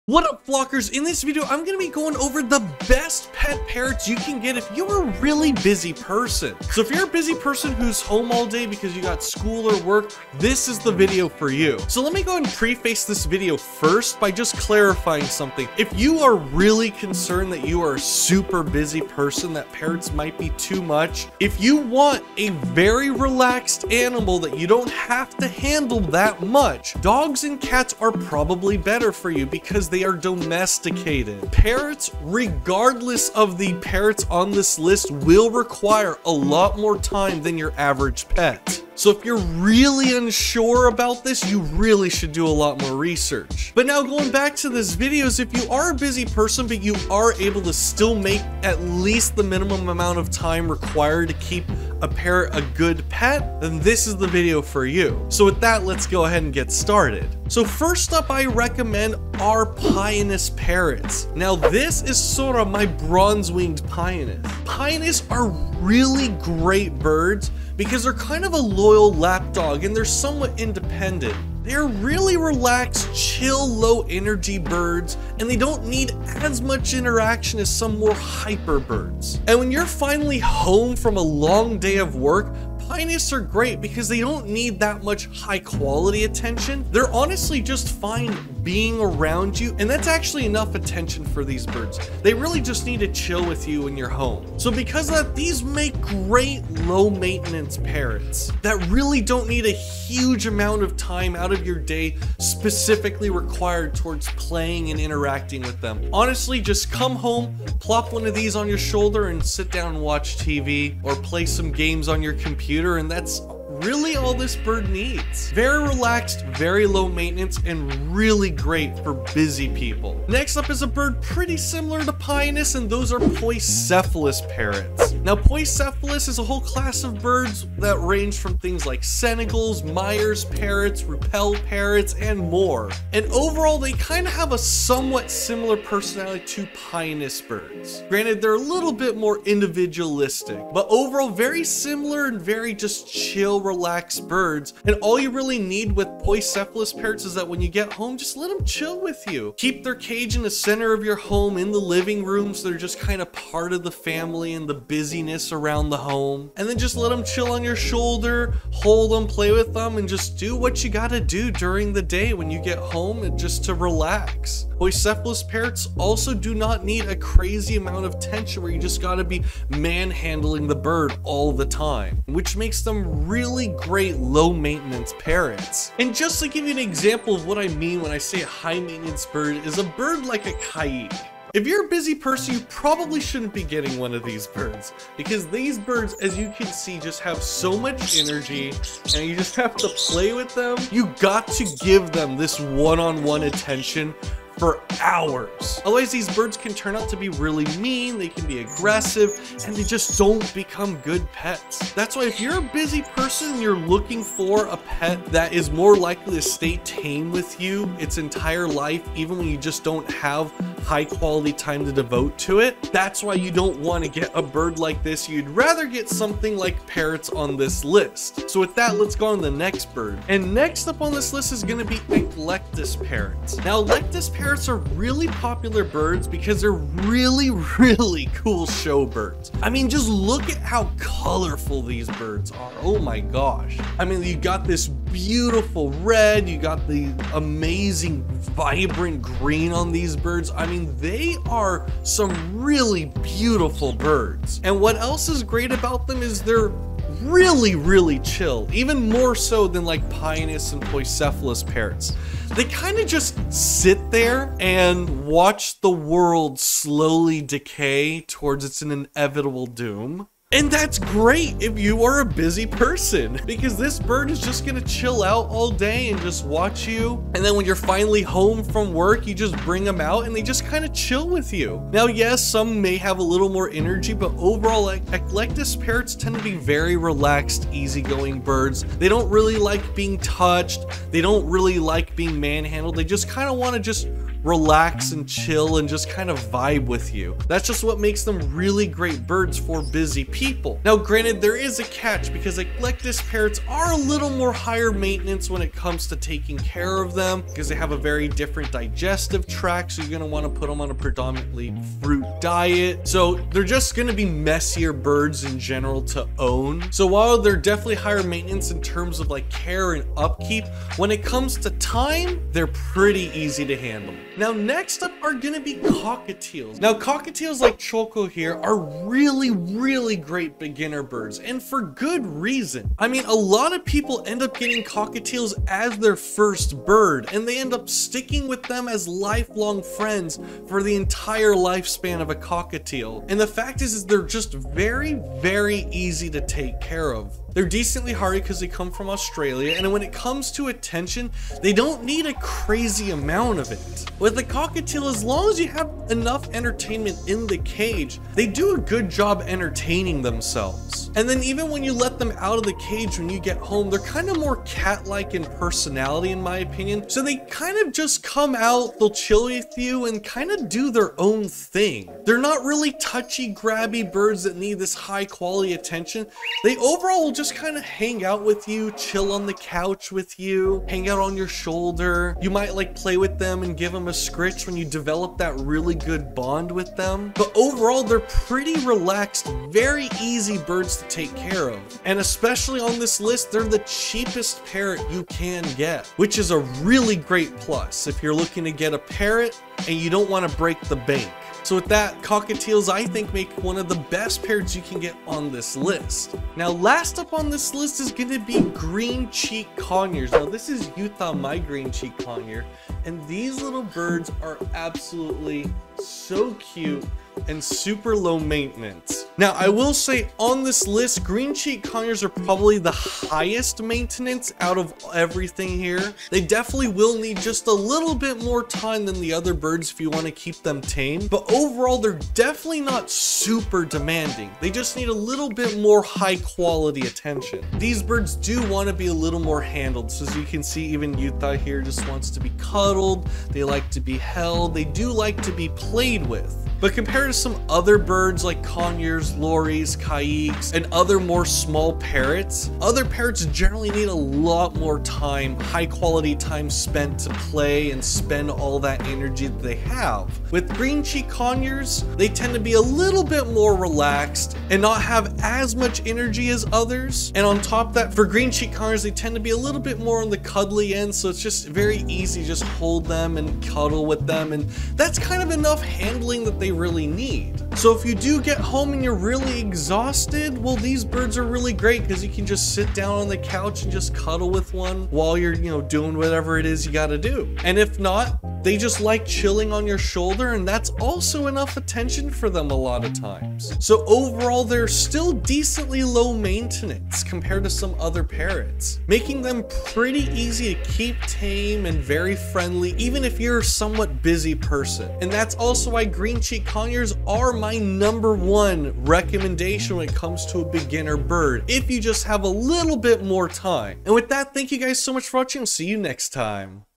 Top Pet Parrots For Busy People
If you're a busy person but also still wanting a pet parrot, these are my recommendations on the best pet parrots money can buy.

Bird Stuff!
Best Small/Medium Parrot Food
Best Large Parrot Food
Table Perch